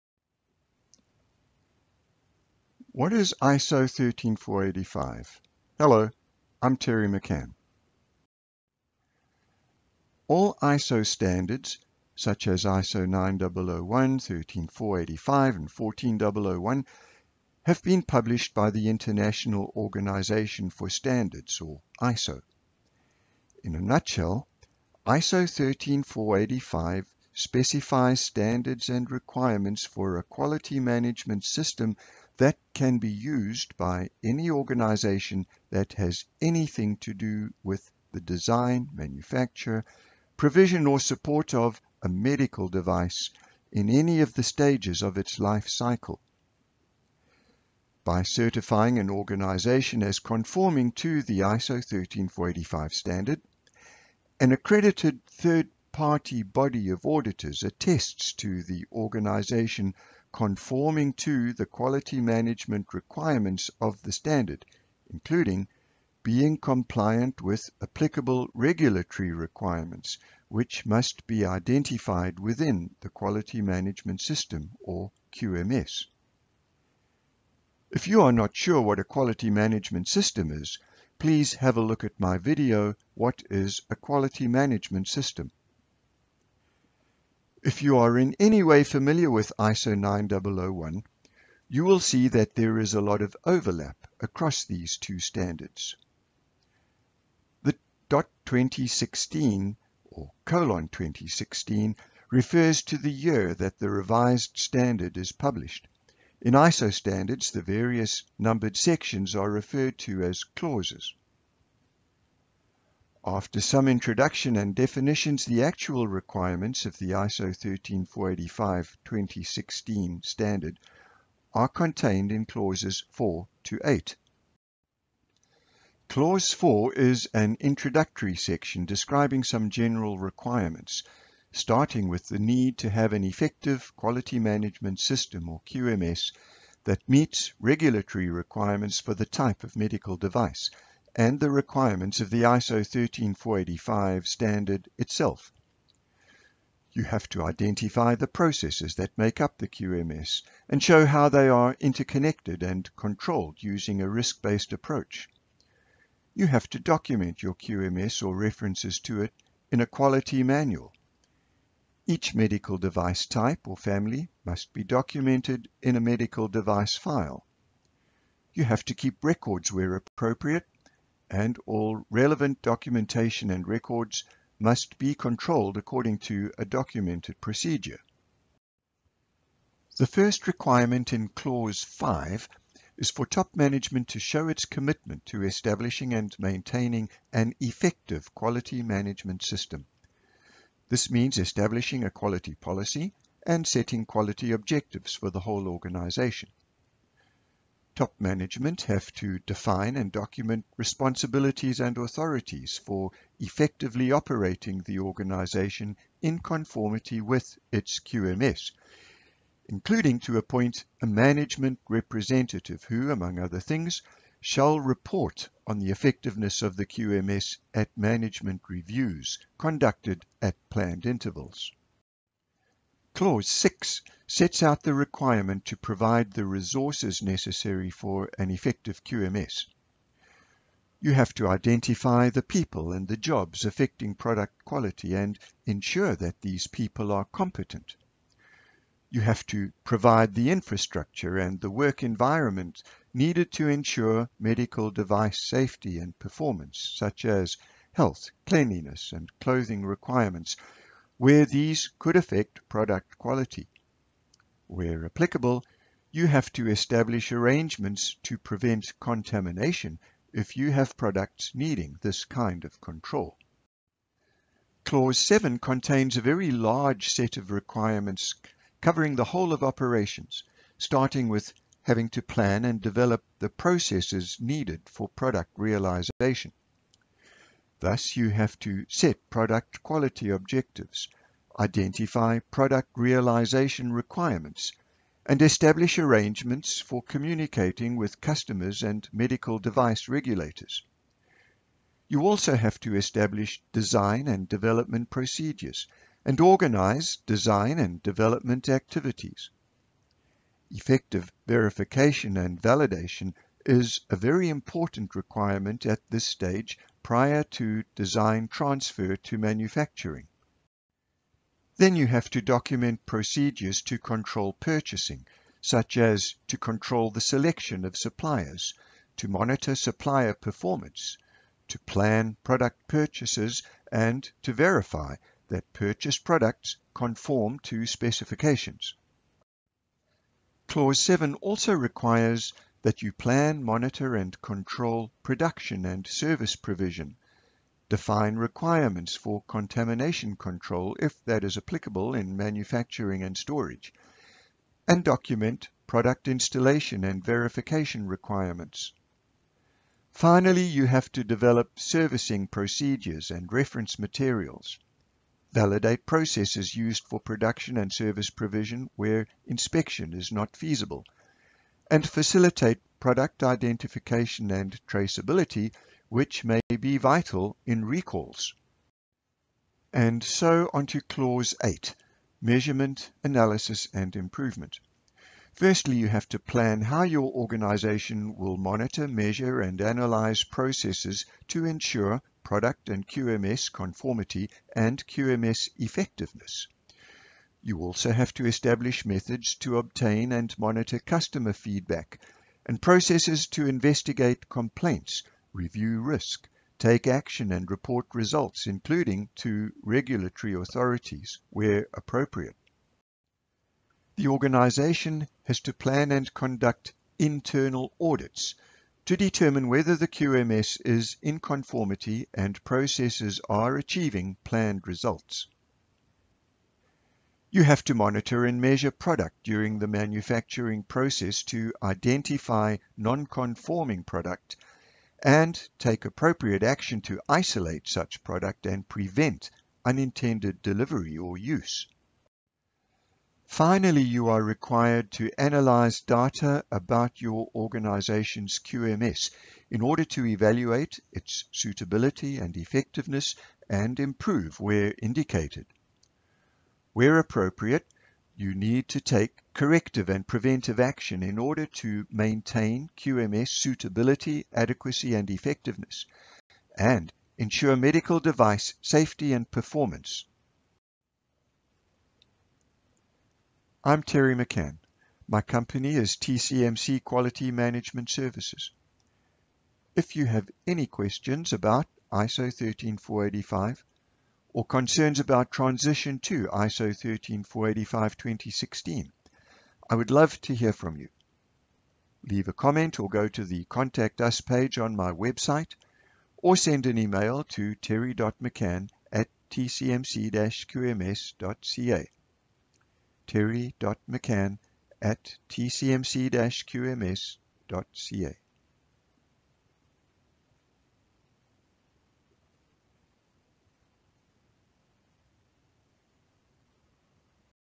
What is ISO 13485 for medical devices?
A brief introduction to this ISO Standard for medical devices. ISO 13485:2016